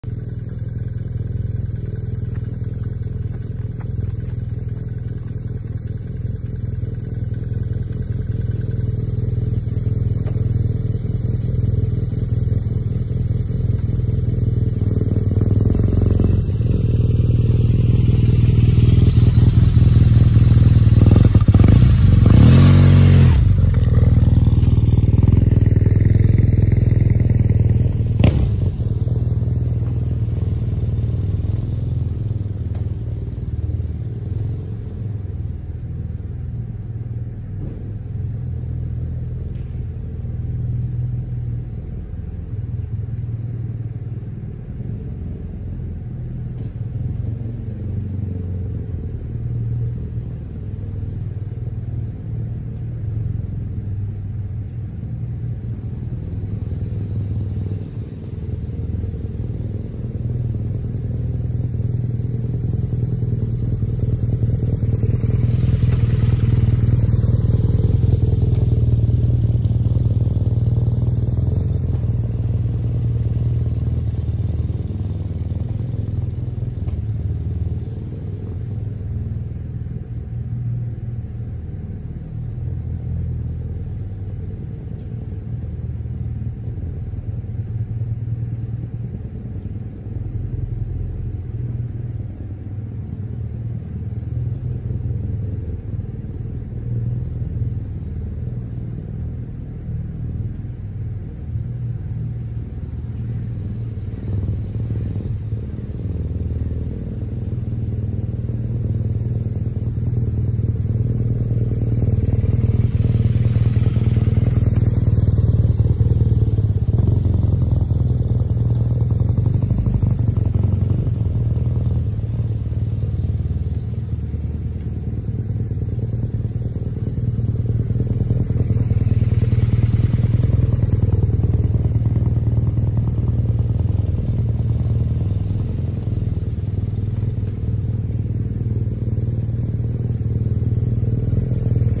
หัดขับใหม่ ใจสังมา
วีธีขับรถbig bike หรือ มอนส์เตอร์ไซร้ 1000 แรงม้าขึ้นไป
แบบหัดขับช่าๆ ประค้องรถให้ของครับ